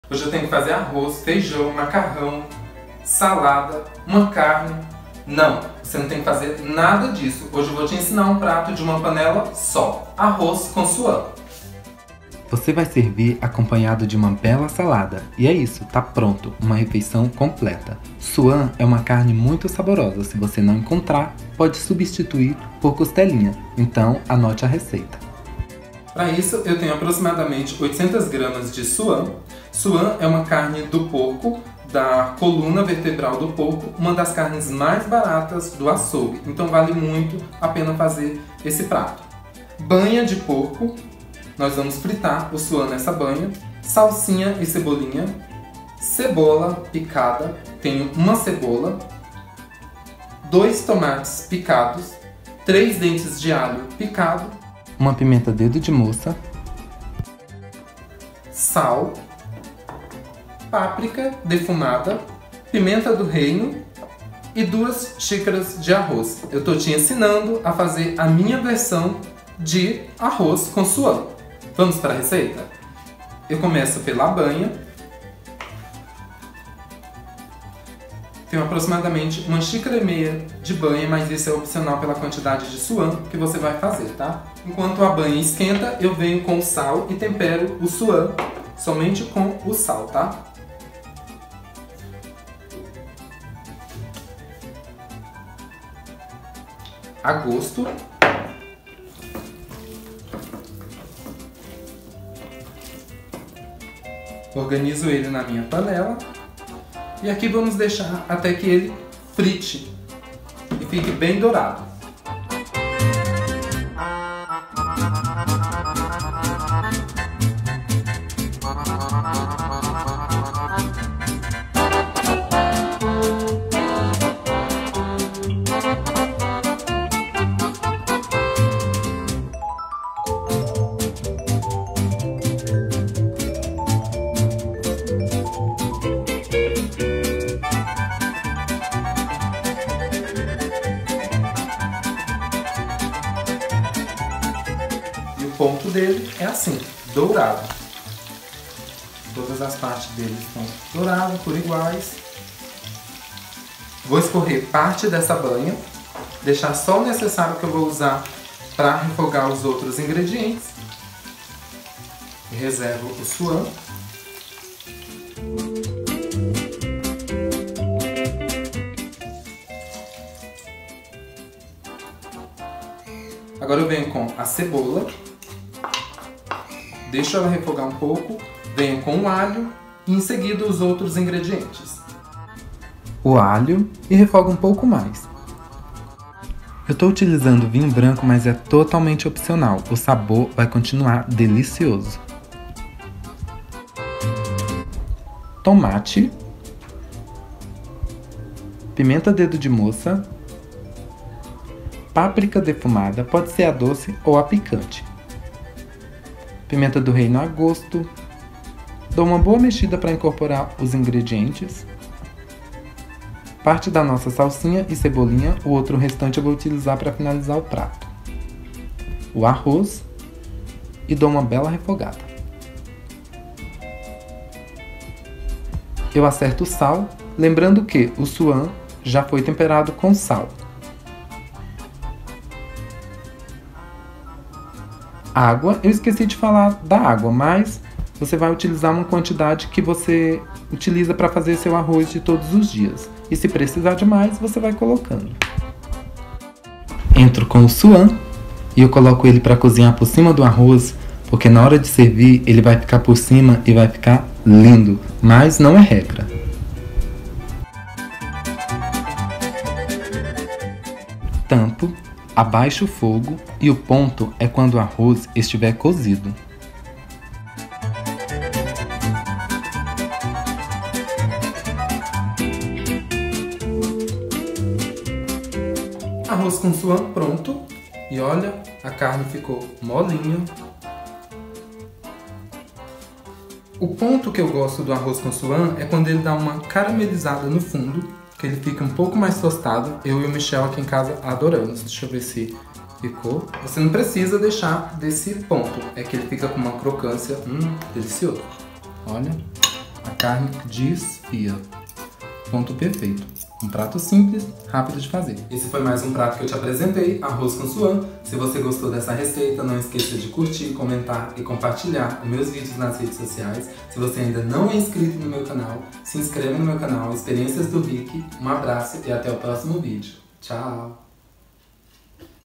ARROZ COM SUÃ !
INGREDIENTES:

- 800g de suã
- 2 xc de chá de arroz
- 1 1/2 xc de chá de banha de porco, azeite ou óleo
- 1 cebola grande ou 2 pequenas picadas
-  2 tomates picados
- 3 dentes de alho picados
- 1 pimenta dedo de moça picada
- 1 colher de chá de páprica defumada, doce ou picante
- salsinha e cebolinha á gosto
- sal á gosto
- pimenta do reino á gosto
- vinho branco totalmente opcional

MODO DE PREPARO:

Assista ao vídeo, que eu ensino o passo a passo certinho.

Aproveita e...

👍🏻 curta
🗣 comenta
🌎 compartilhe
✅ salve essa receita